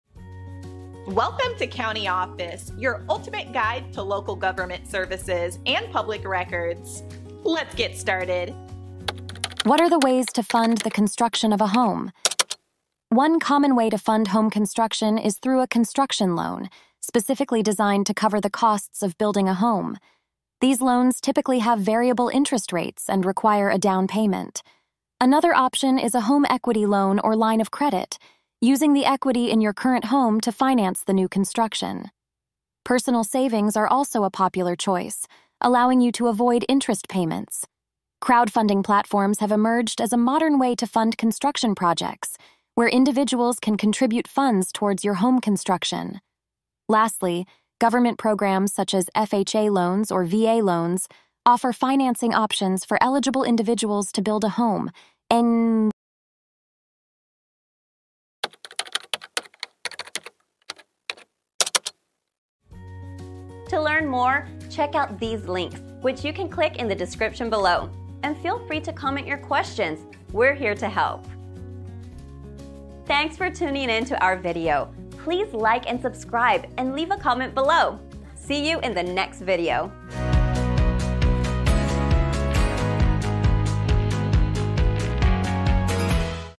What Are The Ways To Fund The Construction Of A Home? - CountyOffice.org
What Are The Ways To Fund The Construction Of A Home? In this insightful video, we delve into the various ways to fund the construction of a home, providing you with valuable information on financing options available for your home building project. Discover the benefits and considerations of utilizing construction loans, home equity loans, personal savings, crowdfunding platforms, and government programs like FHA and VA loans to bring your dream home to life. Learn how each funding method works and determine which option aligns best with your financial goals and circumstances.

Join us for an in-depth discussion on home construction financing strategies and empower yourself with the knowledge needed to make informed decisions about funding your next construction project. Don't miss out on this essential guide to navigating the world of home financing.